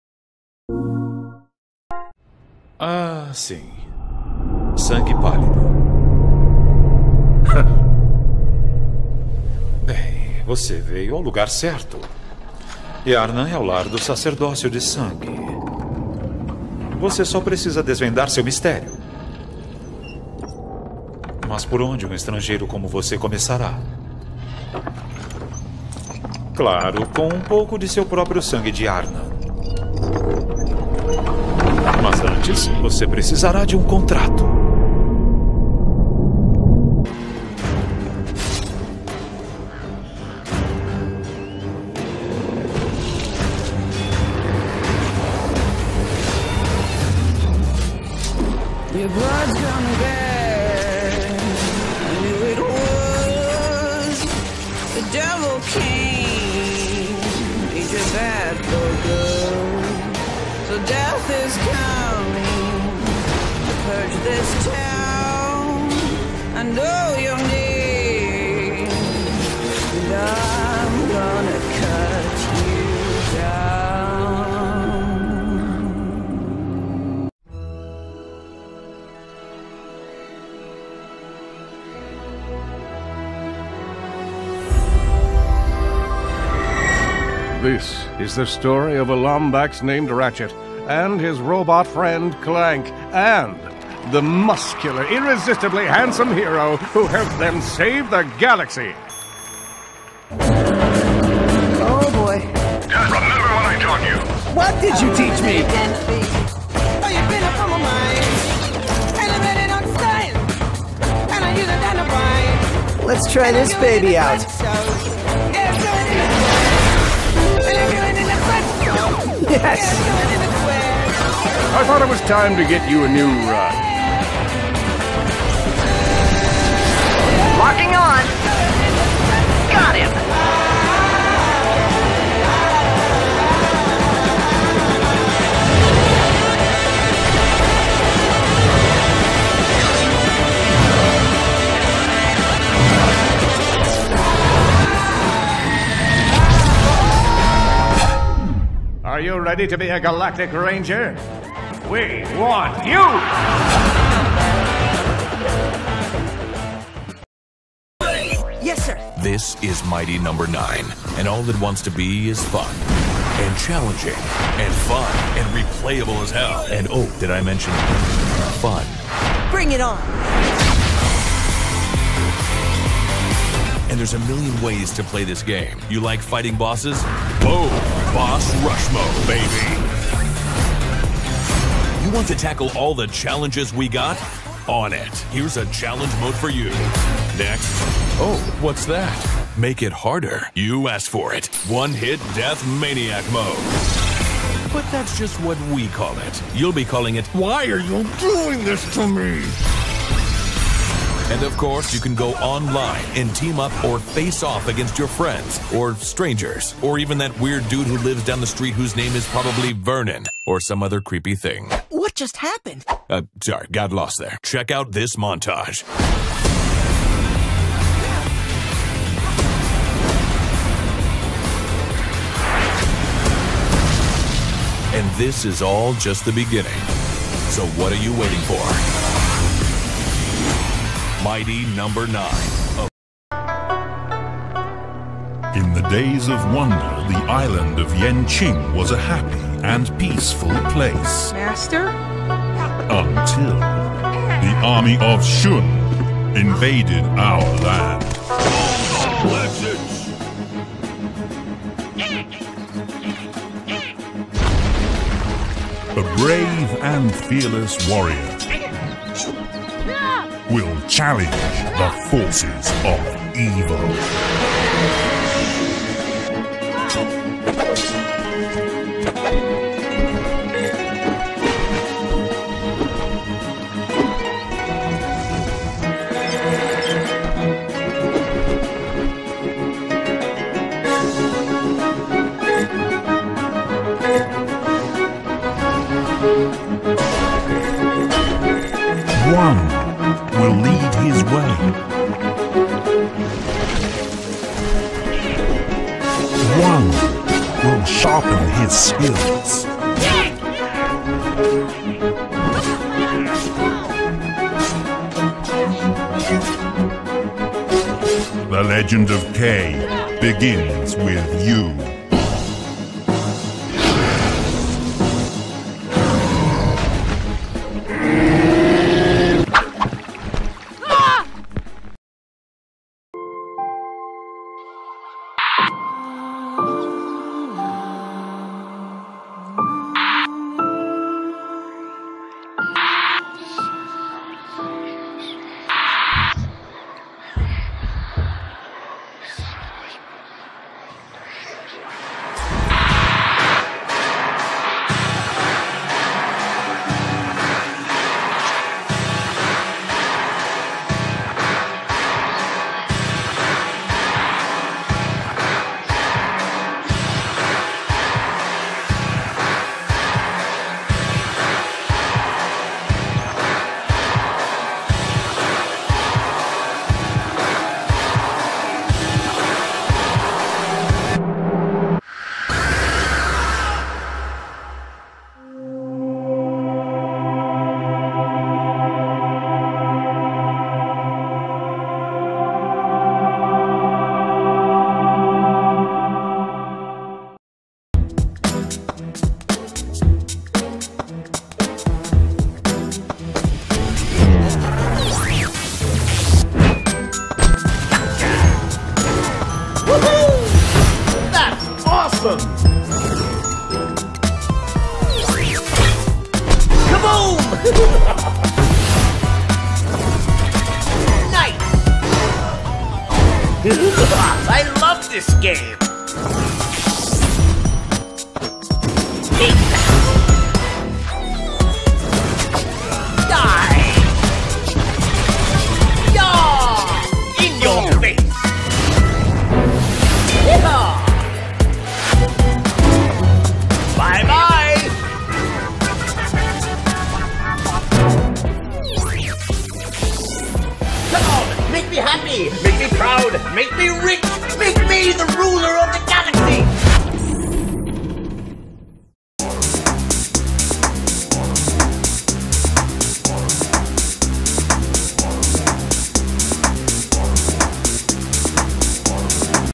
Playstation Plus Março 2018| PS4 | PS3 | PS VITA
Marque o 🔔 ao lado do botão "Inscrito" para receber as notificações.

Confira a lista completa de jogos gratuitos da PS Plus de Março 2018:

Bloodborne (PlayStation 4)
Ratchet & Clank (PlayStation 4)
Mighty no. 9 (PlayStation 3 e PlayStation 4)
Legend of Kay (PlayStation 3)
Claire: Extended Cut (PS Vita e PlayStation 4)
Bombing Busters (PS Vita e PlayStation 4)

Os jogos serão disponibilizados na primeira atualização mensal
da loja PlayStation Store, que costuma acontecer na primeira terça-feira do mês.

Então se gostou ou não, dá um tapa lá no "Dedão".
Valeuuu.